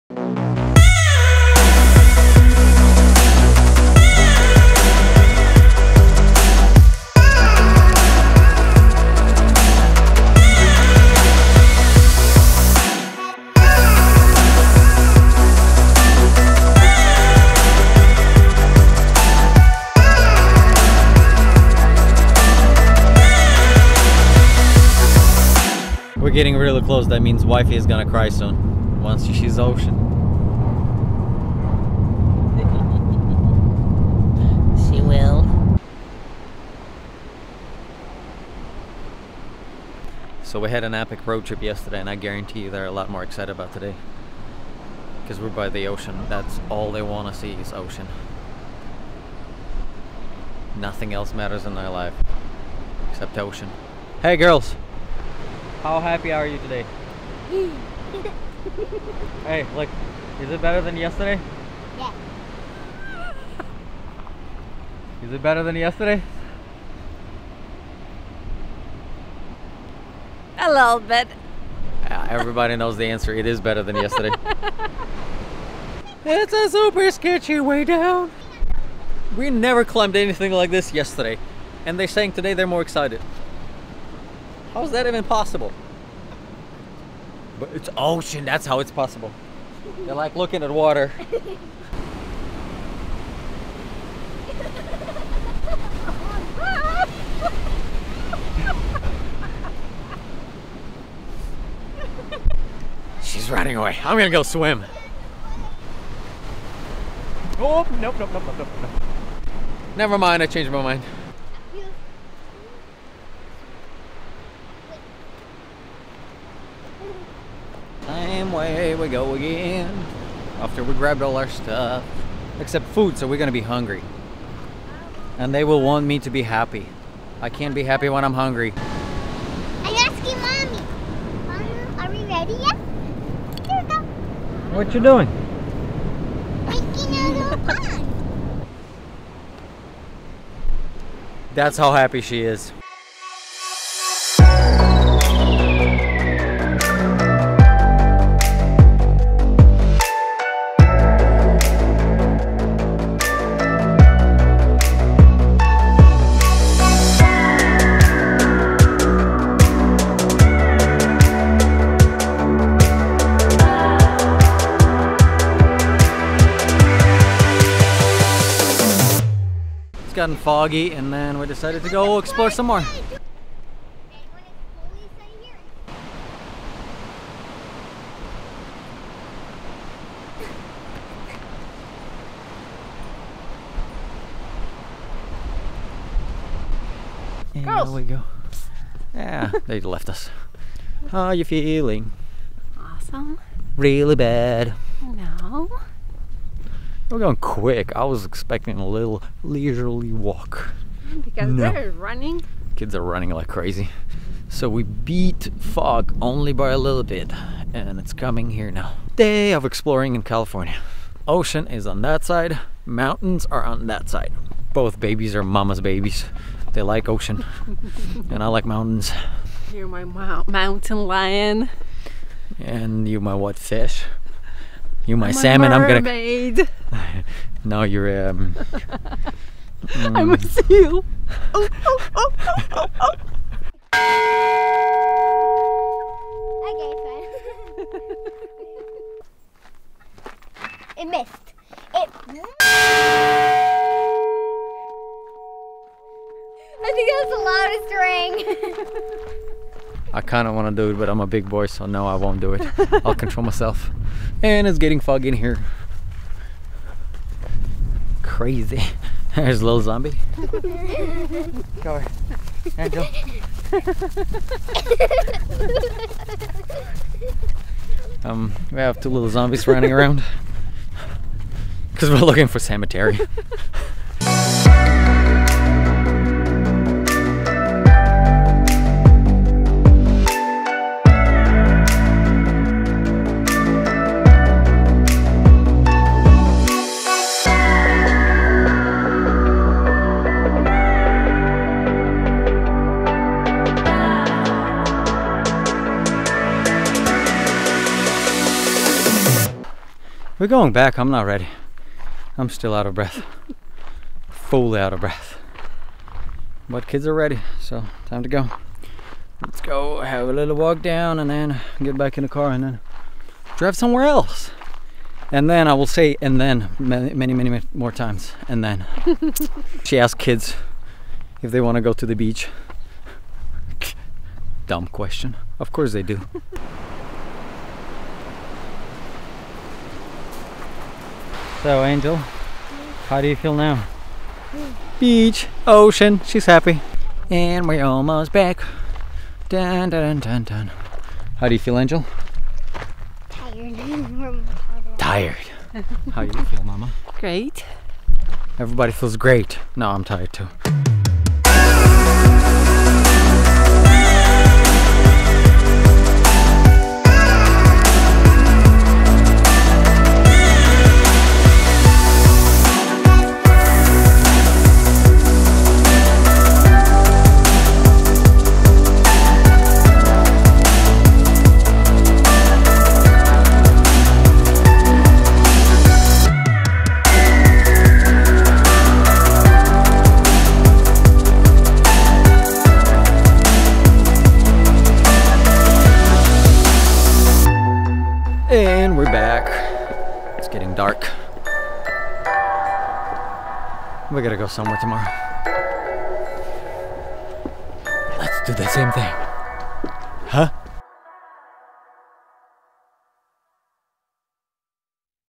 Great Ocean Road Trip is Easy - Vlog on Fujifilm X-T3
After working all week most of the time don't want to do much. Simple choice for us is to go to the ocean. Ocean is our favorite place.